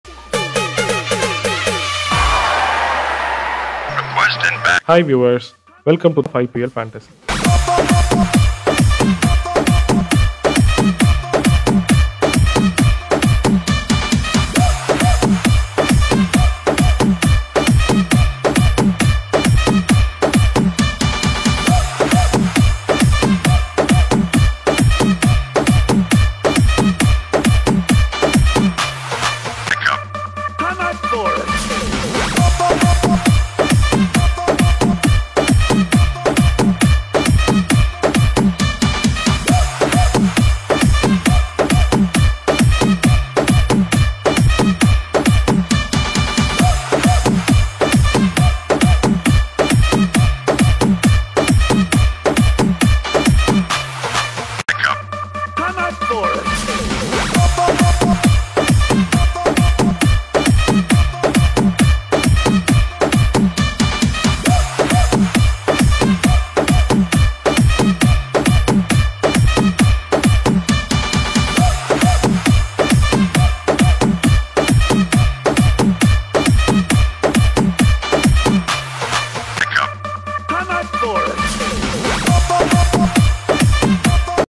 IPL Fantasy: SRH vs RCB Match Prediction | SRH vs RCB Dream11 Prediction
In this video, we bring you our prediction of Match 65: Sunrisers Hyderabad vs Royal Challengers Bangalore

Chapters:
0:00 Introduction
0:05 IPL Fantasy
0:17 Team for Match 65
0:36 Team for Daily Contest
0:57 SRH vs RCB: Who will win?